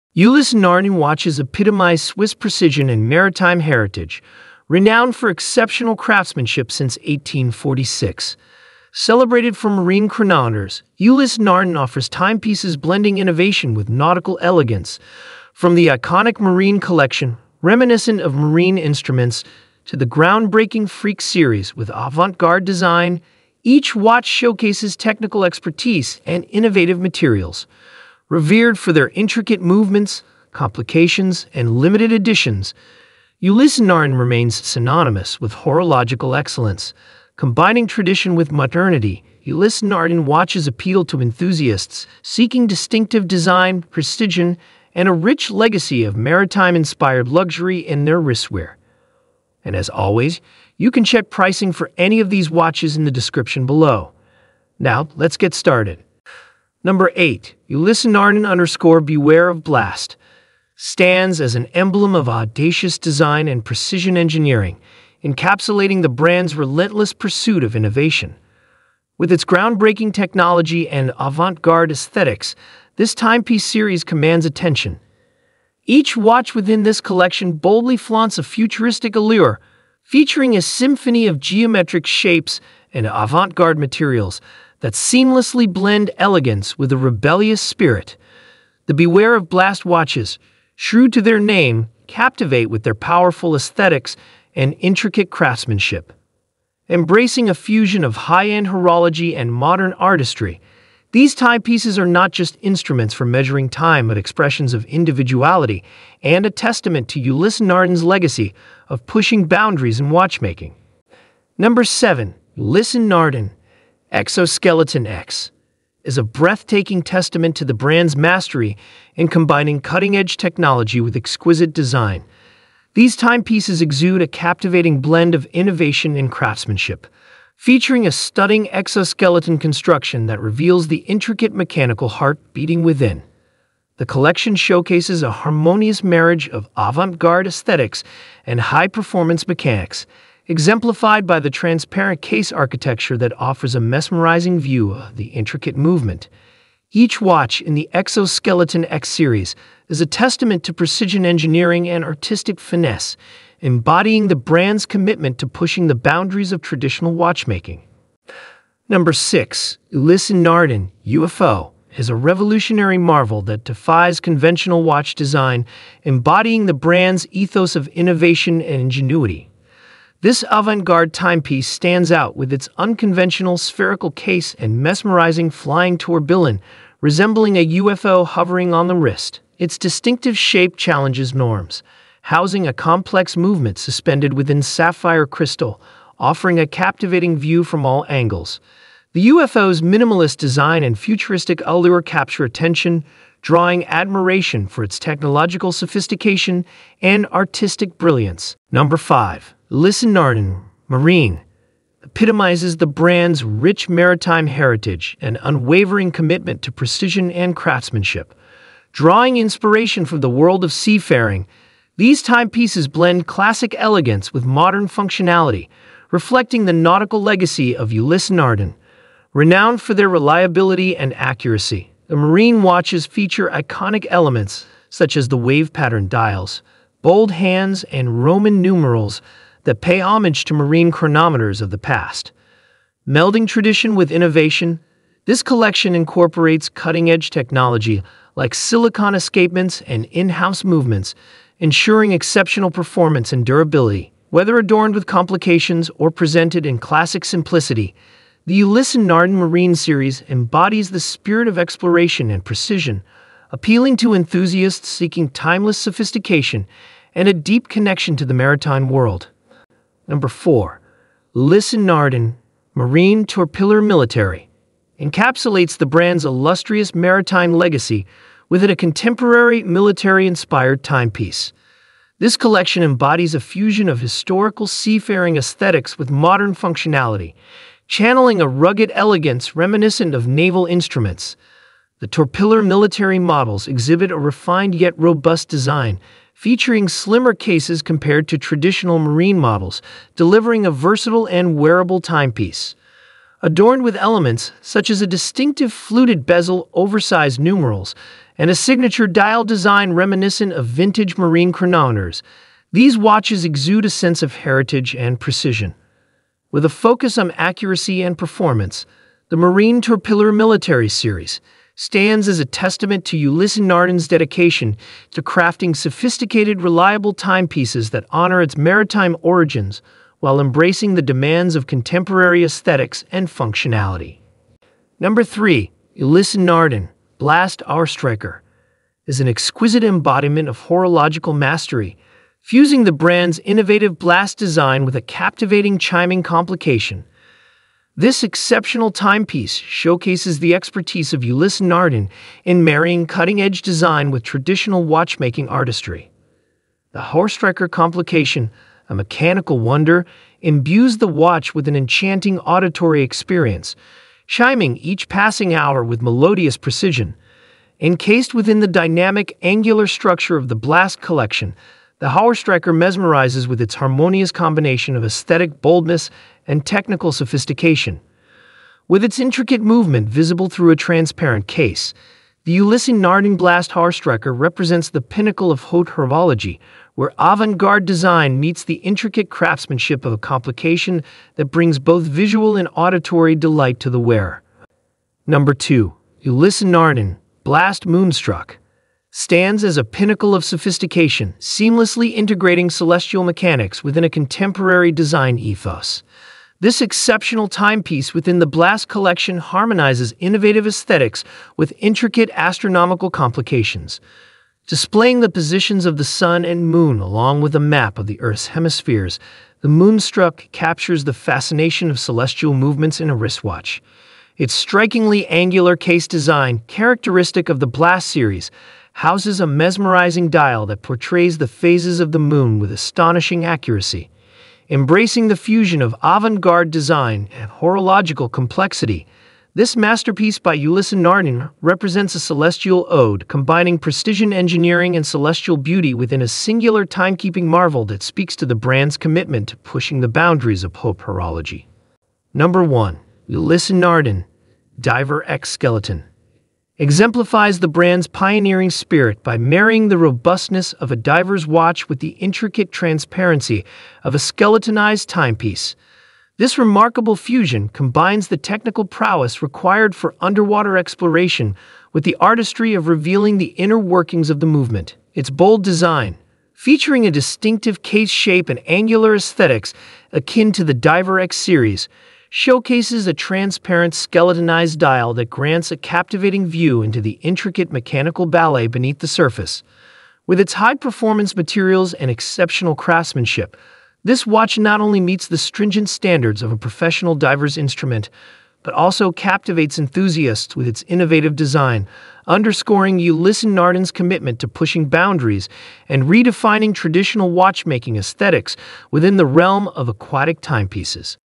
Best Ulysse Nardin Watches 2024: You Can't Ignore!
Links to the Best Ulysse Nardin Watches 2024 are listed below. At Watch Realm, we've researched the top 8 Best Ulysse Nardin Watches 2024 on the market, saving you time and money.

➤ 10. Ulysse Nardin Freak NeXt

➤ 9. Ulysse Nardin - Diver X Nemo Point

➤ 8. Ulysse Nardin Beware of Blast

➤ 7. Ulysse Nardin - Exo-Skeleton X

➤ 6. Ulysse Nardin - UFO

➤ 5. Ulysse Nardin -Marine

➤ 4. Ulysse Nardin - Marine Torpilleur Military

➤ 3. Ulysse Nardin - Blast Hourstriker

➤ 2. Ulysse Nardin - Blast Moonstruck

➤ 1. Ulysse Nardin - Diver X Skeleton